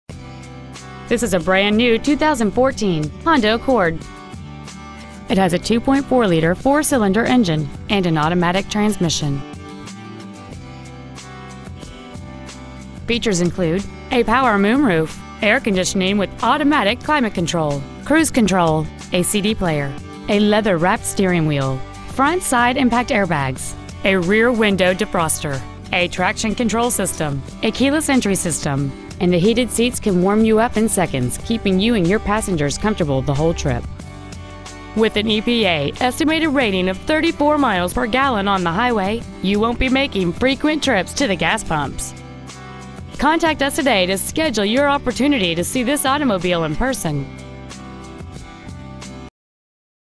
2014 Honda Accord San Jose CA 95129
We've been honored to serve the San Jose CA area , we promise that your experience at our dealership will exceed your expectations ! Year : 2014 Make : Honda Model : Accord Engine : 2.4L I-4 cyl Trans . : continuously variable automatic Exterior : Polished Metal Miles : 2 Interior : Black Honda of Stevens Creek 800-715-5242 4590 Stevens Creek Boulevard San Jose , CA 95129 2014 Honda Accord San Jose CA